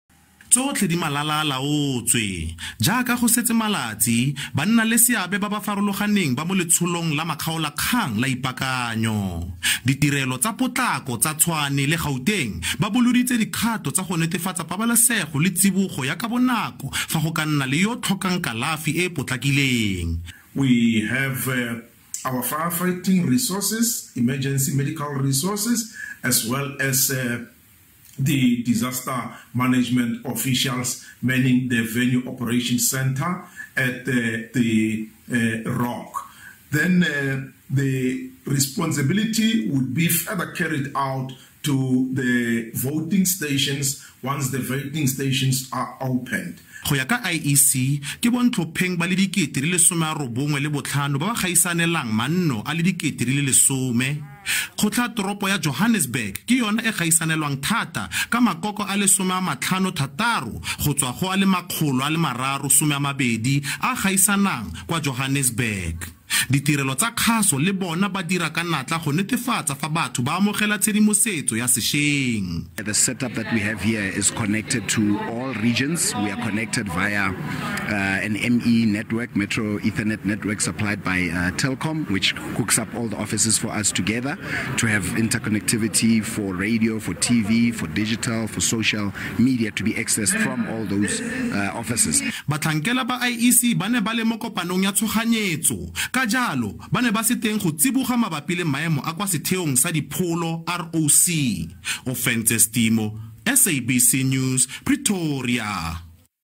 Tikwatikwe ya thebolo ya dipholo tsa ditlhopo e tsweletse ka ipaakanyo
Go simolola tlhanaselo kwa tikwatikweng ya dipholo tsa ditlhopo kwa Pretoria, jaaka dipaakanyo tsa metsotso ya bofelo di tswelela. Batlhankela ba ditirelo tsa tshoganyetso le bona ba rebotseb thulaganyo ya bona.

For more news, visit sabcnews.com and also SABCNews SABCDikgang COVID19News on Social Media.